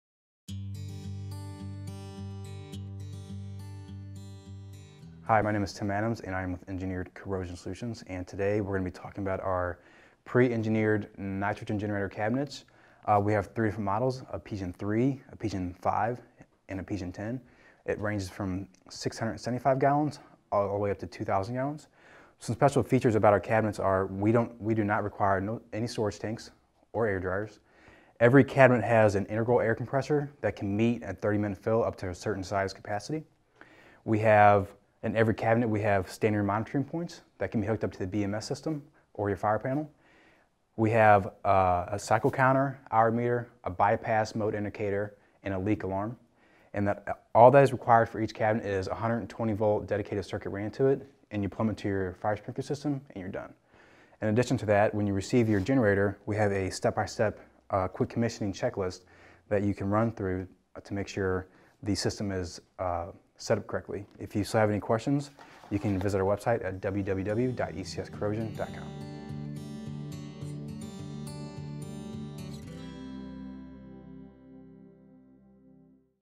Product Demo: Wall-Mount Nitrogen Generators (PGEN 3/5/10)
Existing buildings and small dry or preaction systems require a corrosion management system that works with you at fighting nitrogen production corrosion.

Don’t sacrifice quality for compact size – get up to 2,000 gallons of capacity in a generator the size of an electrical panel. Monitor your generator with the built-in contacts. Forget the air dryer and the storage tank – when your generator produces ultra-dry nitrogen on-demand, why waste valuable floor space when you can buy one of these space-saving wall-mount nitrogen generators.

FM Approved and UL 508A Listed, our equipment meets all applicable standards and is fully compatible with both new and existing nitrogen generator systems.

For use under U.S. Patents 8,720,591, 9,144,700 and 9,186,533